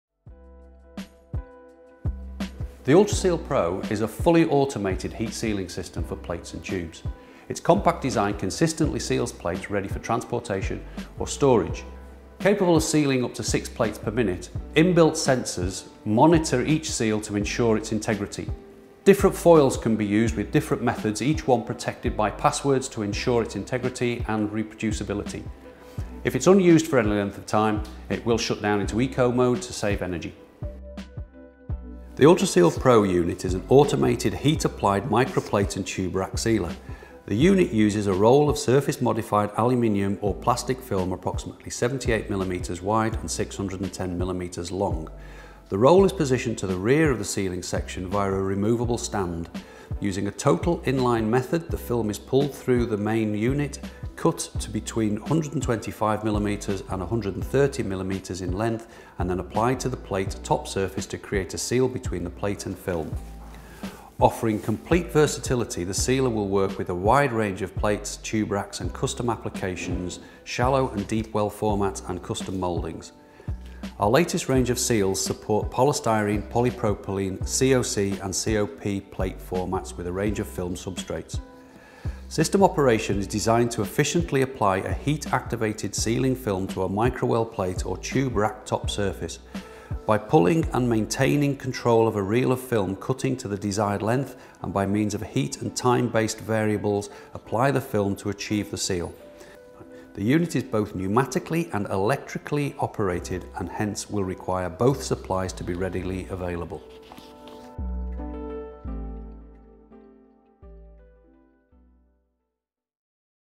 Southern Labware JG Finneran Porvair Ultraseal Pro
Ultraseal Pro – a fully automated heat-sealing instrument designed to eﬃciently seal a wide range of samples of microplates and tubes.